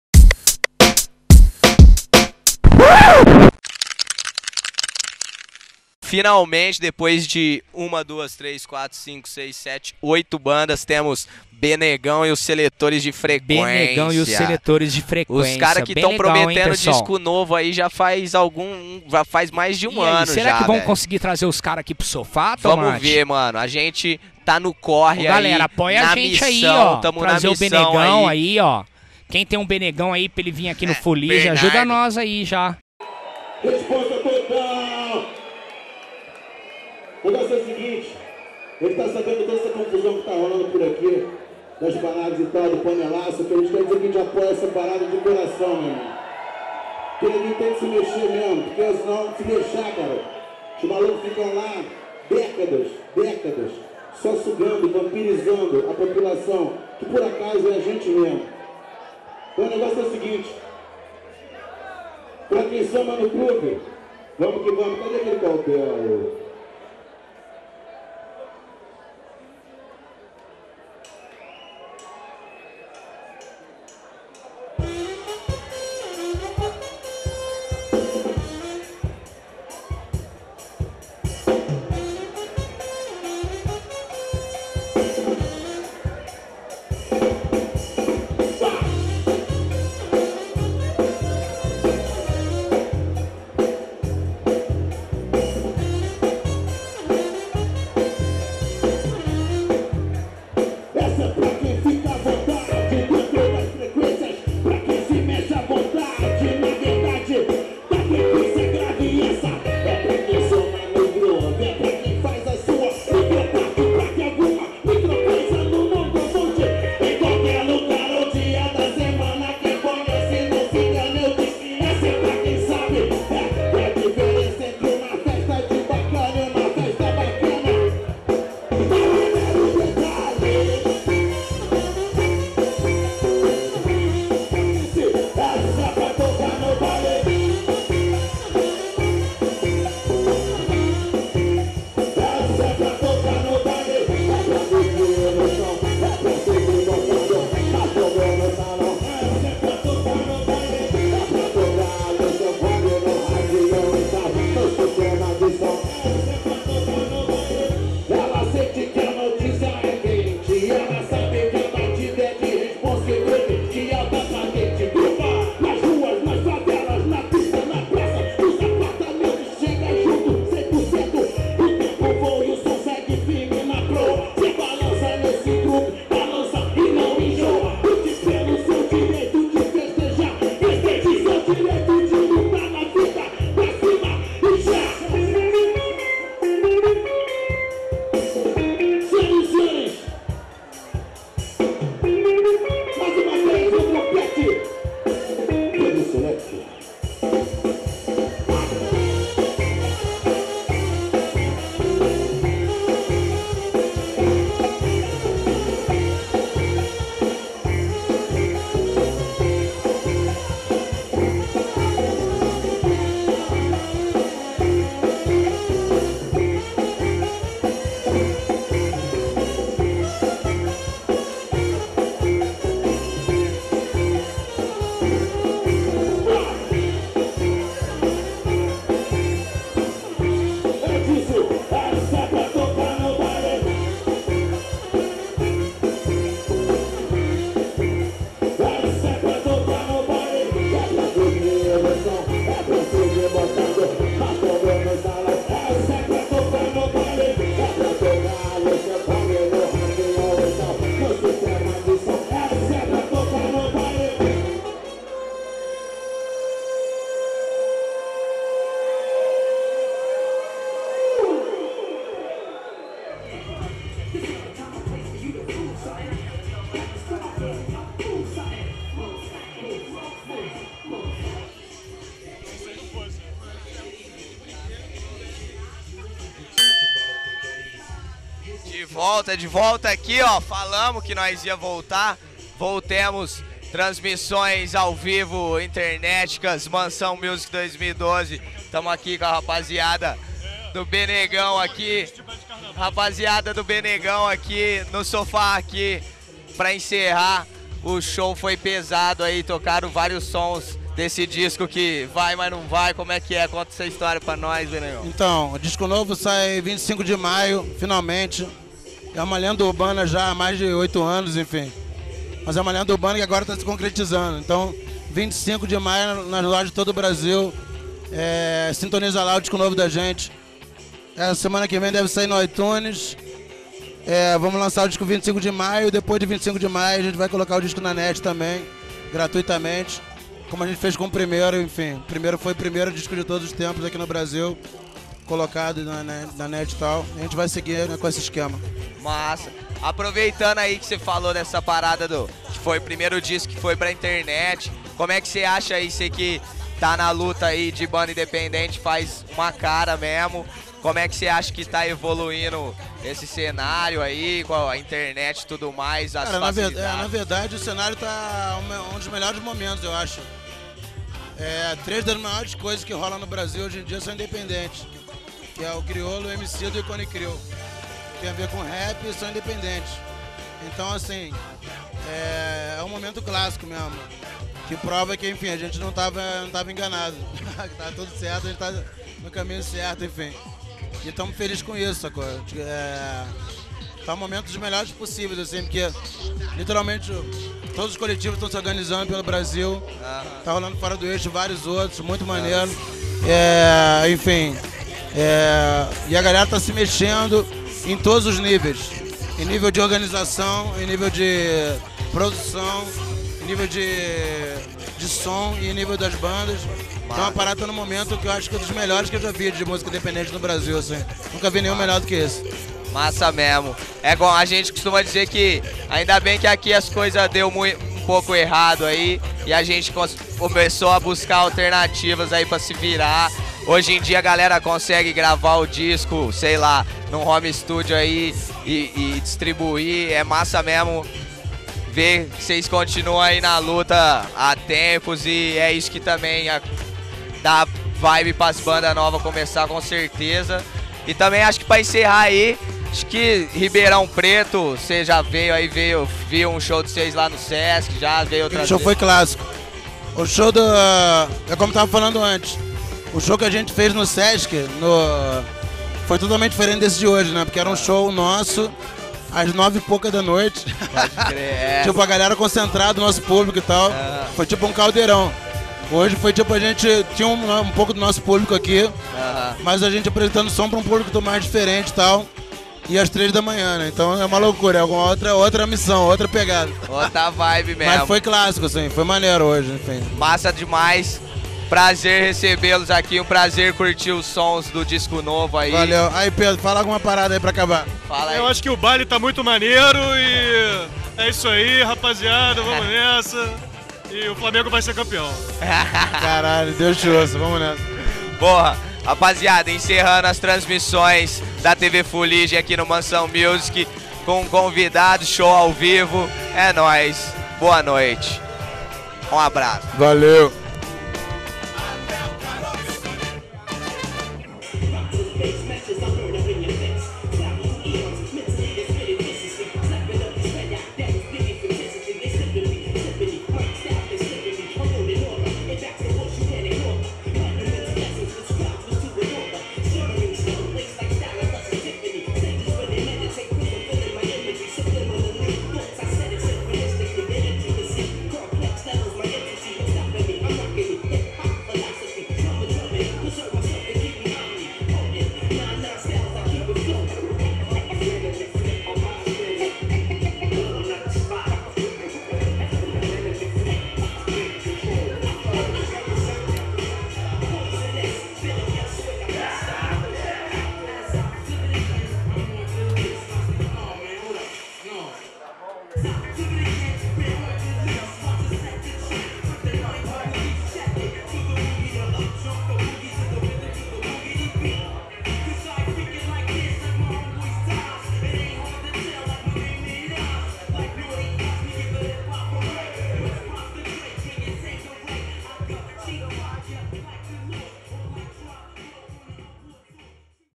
Mansão Music 2012 - Entrevista Bnegão
00:32 - Bnegão comenta o Movimento Panelaço
04:30 - entrevista com BNEGÃO.

A TVFuligem realizou a transmissão ao vivo do evento na integra,
ao total foram mais de 7 horas no ar.
Aqui você confere a entrevista do Bnegão e um pouco do show que aconteceu na Mansão Galo Bravo / Ribeirão Preto.
Atrações:
Senhor X, Chic Hernandes & Batuqueira Band, RP3, Sol de Papel, O Rappa Cover, Dona Benta, Chavala Talhada, Elephant Club, BNegão & Seletores de Frequência e DJ Selecta Robson.